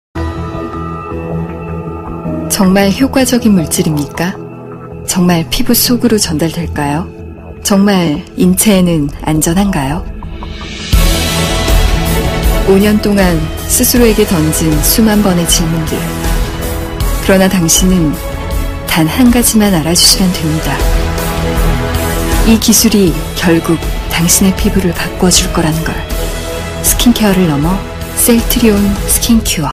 CELLTRION Skincure - Hanskin
Sonunda Hanskin'i Türkiye'ye  tanıttığımız için çok mutluyuz!
Orijinal K-beauty markalarından biri olan Hanskin, yüksek kaliteli formülasyonları ve lüks ürün deneyimleriyle tanınır. BB kremleri Kore'de ilk başlatan ve popülerleştiren marka olarak kabul edilen bir Hanskin ürününü denediğinizde kalite ve yenilik elde edeceğinizi bilirsiniz.

Hanskin bir CELLTRİON markasıdır.